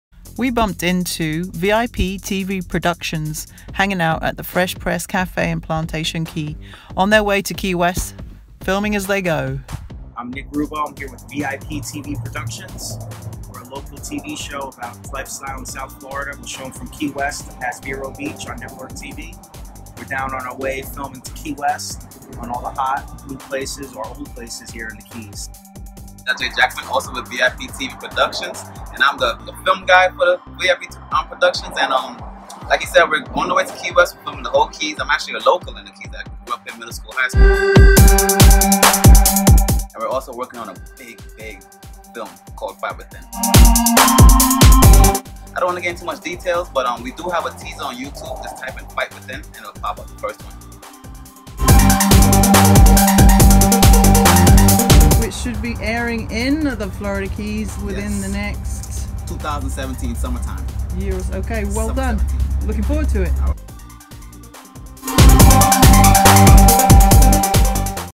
VIP TV PRODUCTIONS fresh press cafe
VIP TV Productions Fresh Press Cafe run in with a random act of Florida Keys social media on an autumn Friday morning in Islamorada. Discover Jamckam in the summer 2017 release teaser trailer on youtube/fightwithin